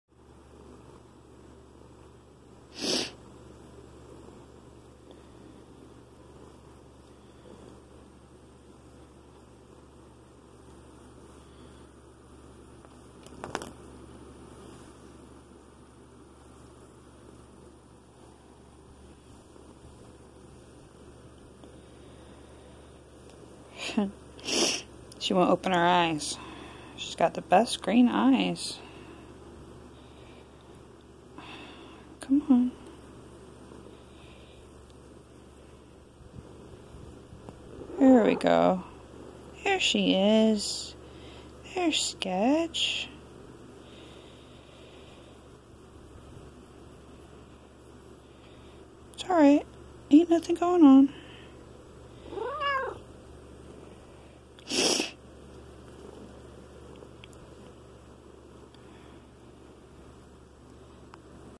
Sketch my 11 year old black cat getting pets
Rare sighting of my black scaredy cat Sketch getting pets.